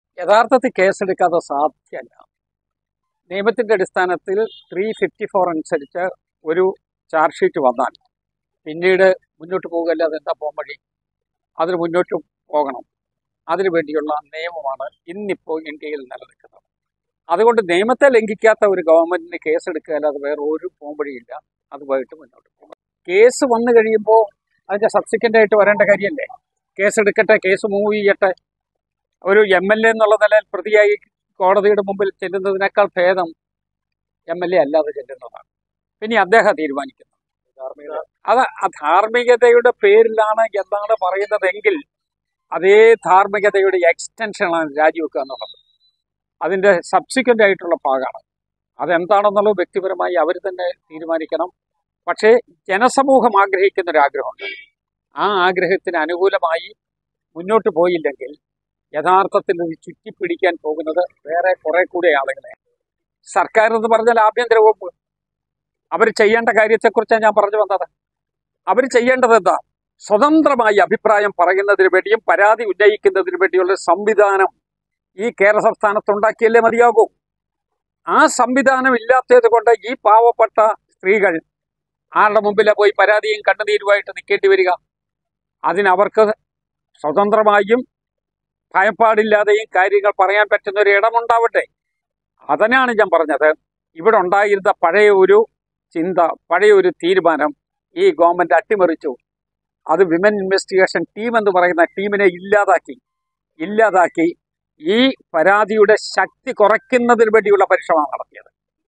മുകേഷ് എംഎൽഎ സ്ഥാനം രാജിവച്ചു കോടതിൽ പോവുന്നതാണ് ഉചിതം || തിരുവഞ്ചൂർ രാധാകൃഷ്ണൻ
പരാതി വന്ന സ്ഥിതിക്ക് കേസ് എടുത്തു മുൻപോട്ടു പോവാൻ മാത്രമേ സാധിക്കൂ എന്ന് തിരുവഞ്ചൂർ രാധാകൃഷ്ണൻ.

 കോട്ടയത്ത് വാർത്താ ലേഖകരോട് സംസാരിക്കുകയായിരുന്നു  അദ്ദേഹം.